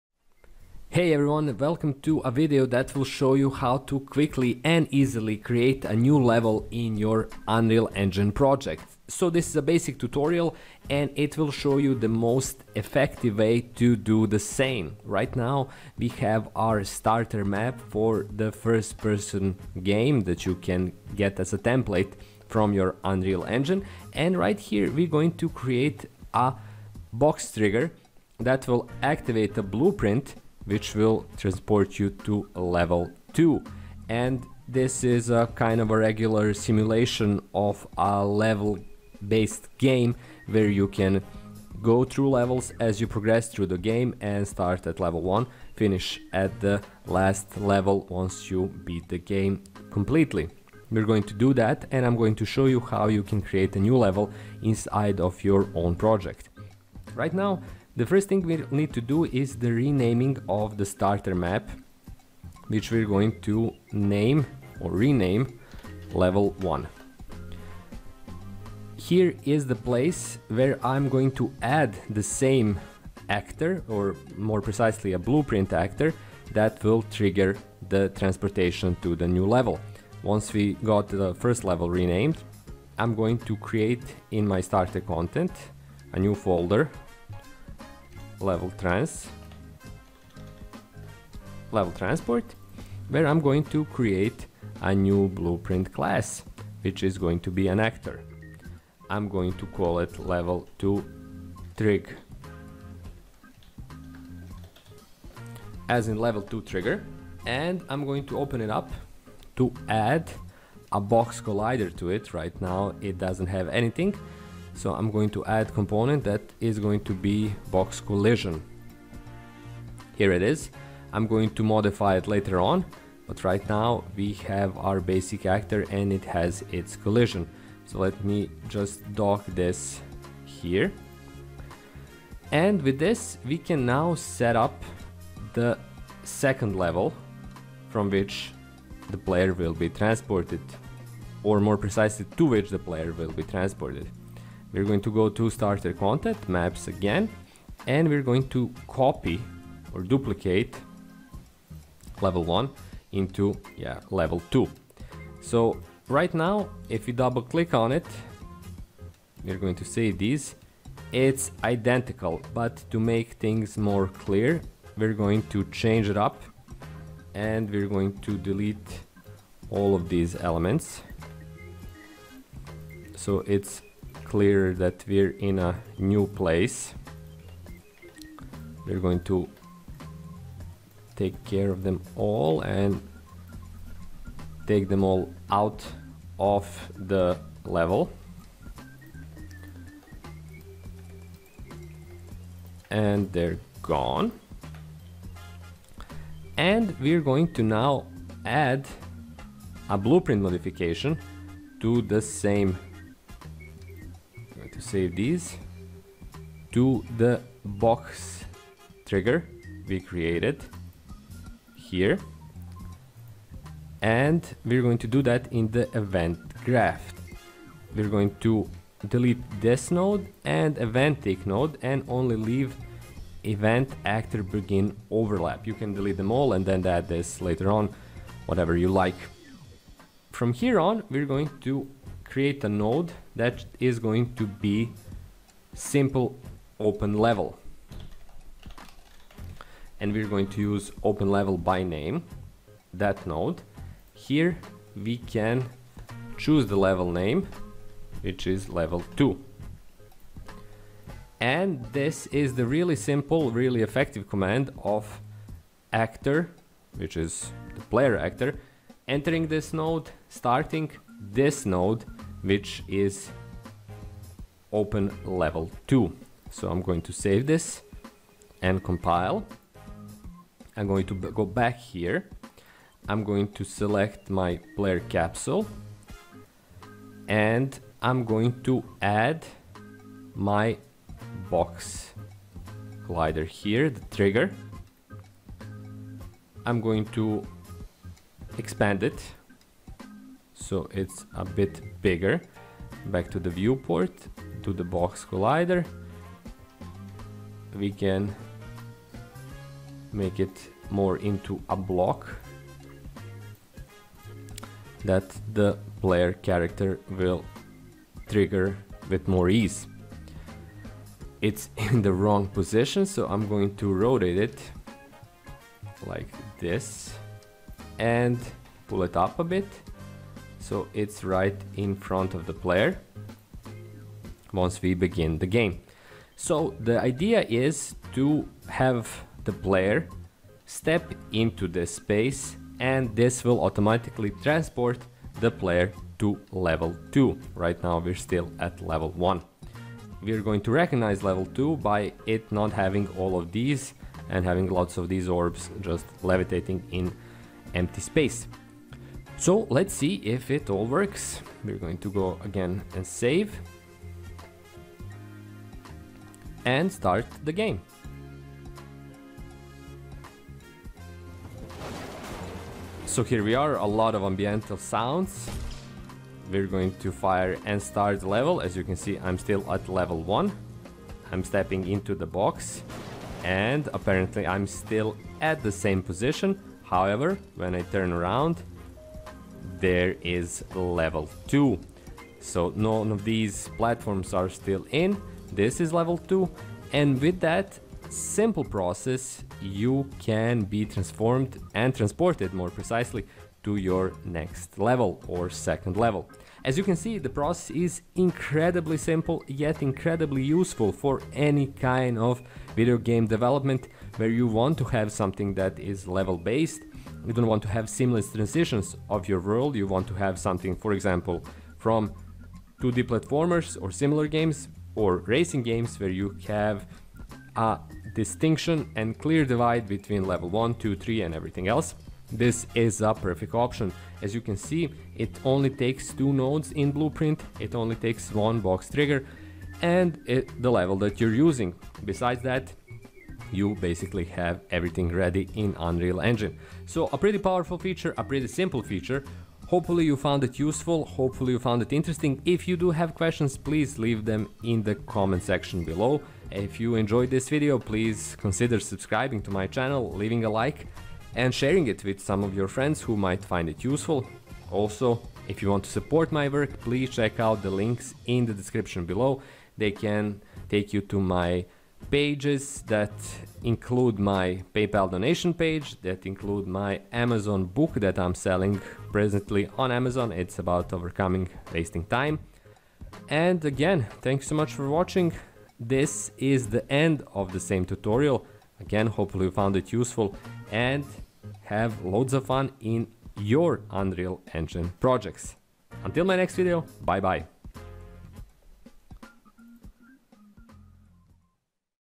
How to Create New Levels in Unreal Engine - UE Beginner Tutorial
Overcome procrastination today!

Hi and thanks for checking out this Unreal Engine beginner tutorial on how you can make new levels in your UE project. This means that you can add a sequence of maps, or levels, to your game. This is the basis of game development in genres like platformers or racing games, all of which have distinct levels and not a seamless world and invisible transitions from one part of the map to the other.

This Unreal Engine new level tutorial will show how these can be added. The process is exceedingly simple and includes first of all creating a new actor blueprint, along with a box collision, which will then begin a simple two-node blueprint action to start an Open Level node. That is everything needed for the same process - once the players enter the collision box they will be taken to the next level. With that, you can learn how you can make new levels in Unreal Engine in a matter of minutes.